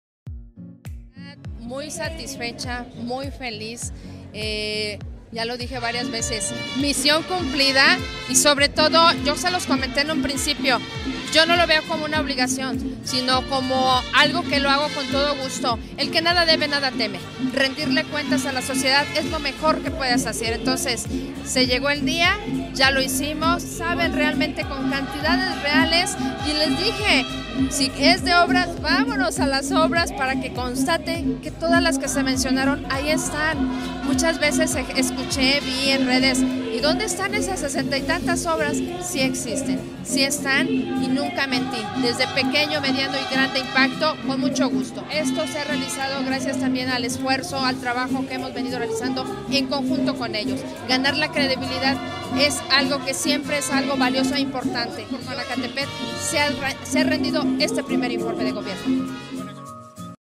RINDE BRENDA GUERRA VALAGUEZ PRIMER INFORME DE GOBIERNO
Acompañada del gobernador del estado, Cuauhtémoc Blanco Bravo, así como de funcionarios estatales, regidores y población en general, la alcaldesa de Jonacatepec de Leandro Valle, Profra. Brenda Guerra Valaguez, rindió su primer informe de gobierno de actividades este viernes 20 de enero.